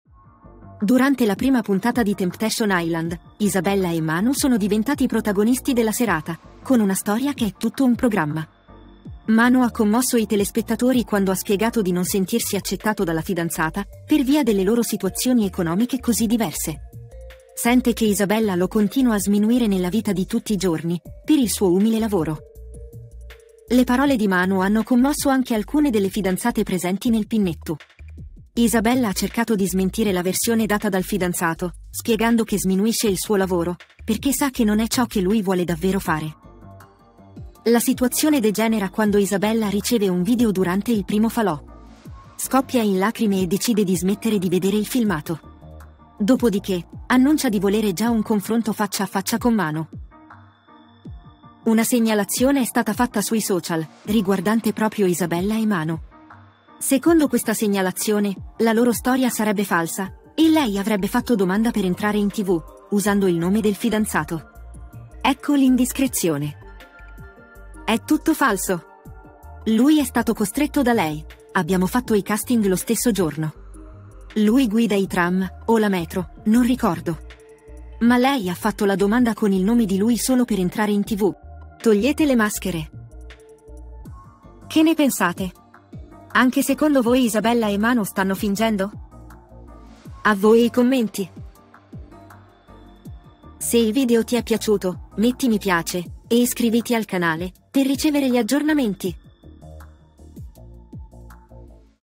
Segnalazione a Temptation Island: La Coppia Sta Mentendo!
Temptation Island è partito alla grande e già arrivano le prime indiscrezioni. Pare che una coppia del reality non sia proprio sincera. Una segnalazione social getta ombra su di loro. Scopriamo insieme di chi si tratta e cosa hanno combinato.